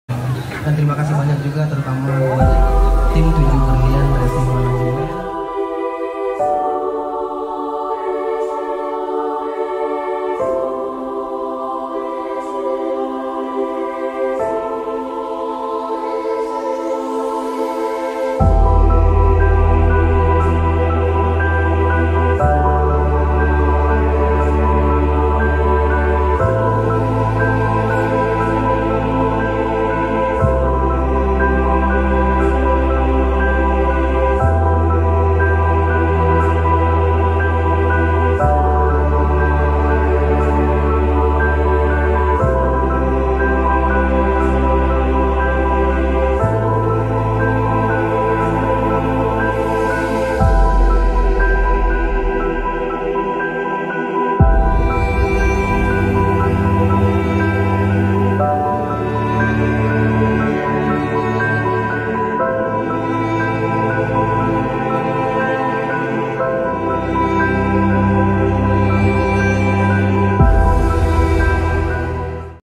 sumatra cup prix
Sircuit permanen satu satu nya d sumatra barat yaitu sawah lunto